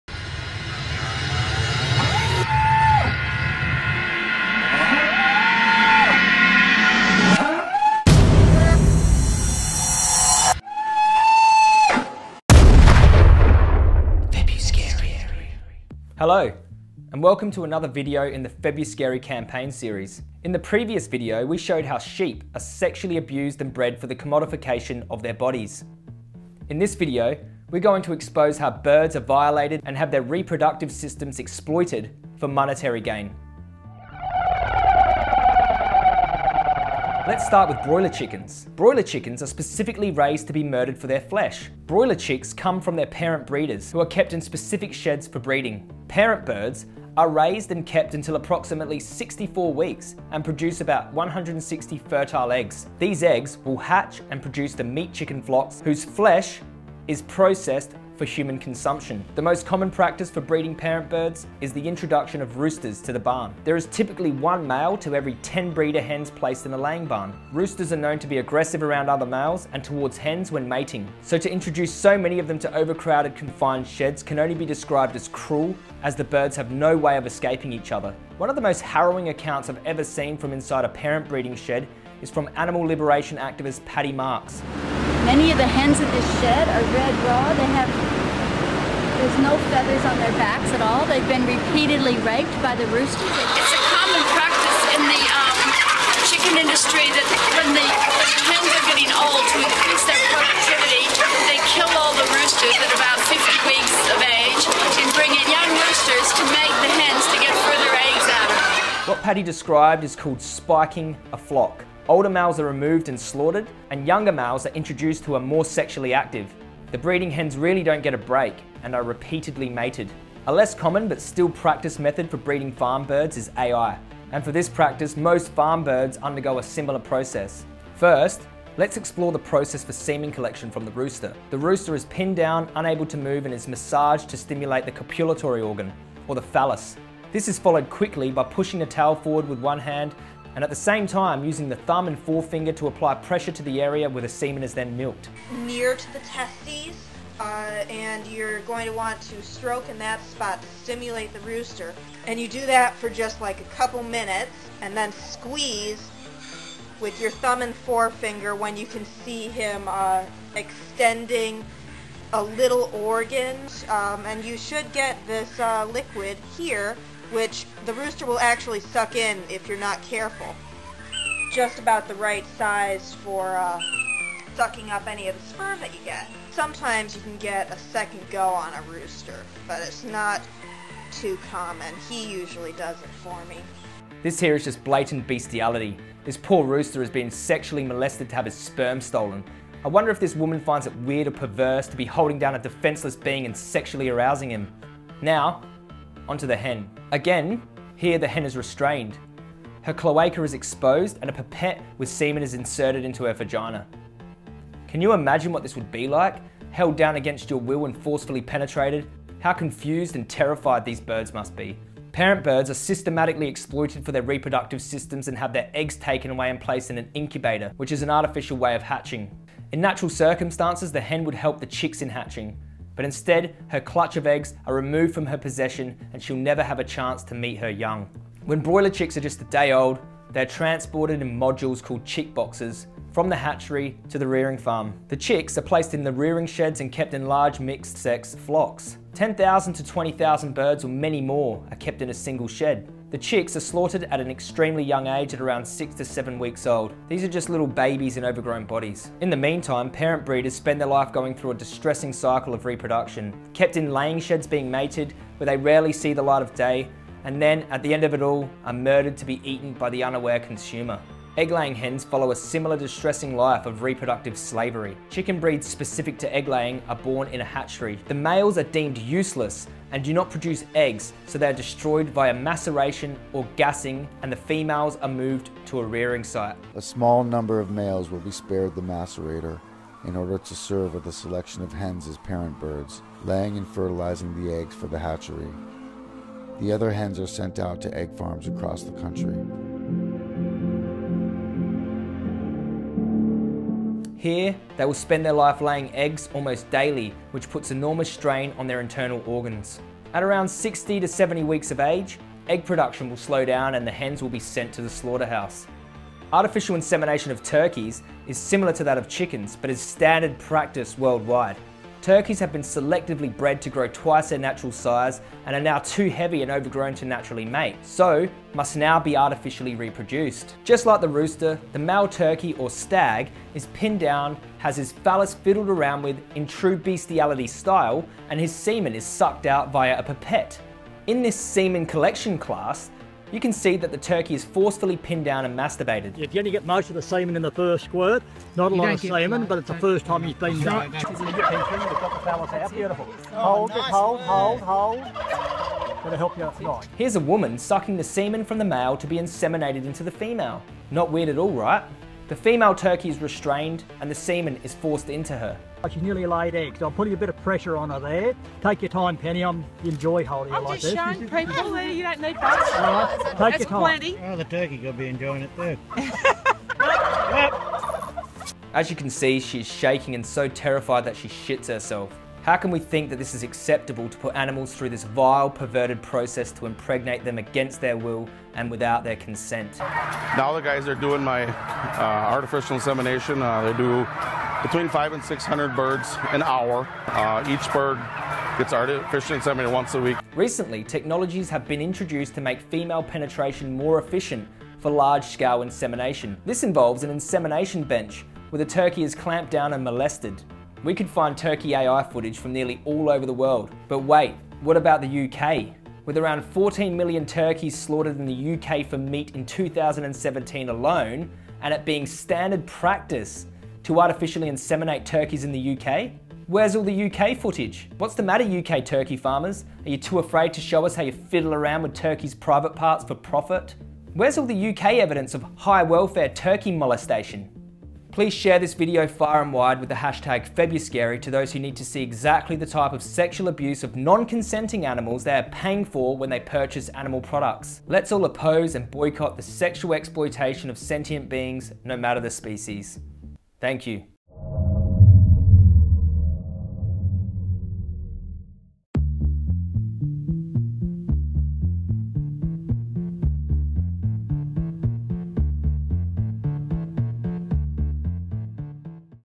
The SCARY Ways Humans Violate BIRDS | FebruSCARY | EP.4
In this video I uncover how chickens, egg laying hens and turkeys are messed around with for animal products

To oppose animal exploitation, be vegan for life, but here's some help to get you started

Filmed: Chris Hines
Project manager: Tarion Partridge